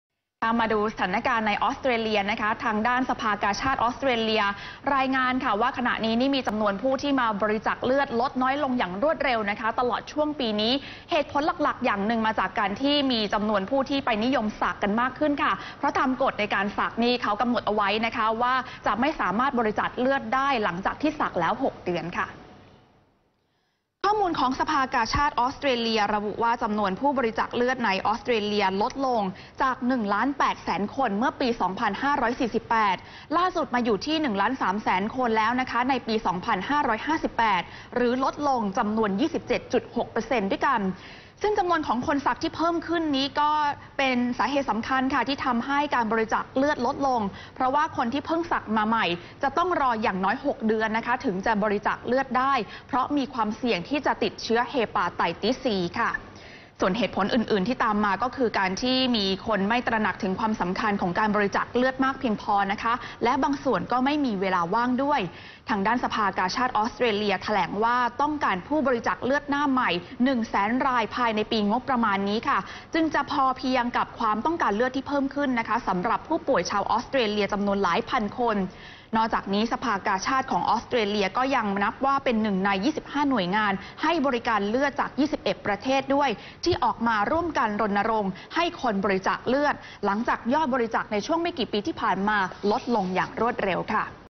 ออสเตรเลียเผยคนสักเยอะ ทำยอดบริจาคเลือดลด
พิกัดโลก ช่วงเช้า : สภากาชาดออสเตรเลียเผยว่า จำนวนคนสักในออสเตรเลียที่เพิ่มสูงขึ้นอย่างรวดเร็วส่งให้ยอดการบริจาคเลือดในประเทศลดลง "NOW26" 18-8-59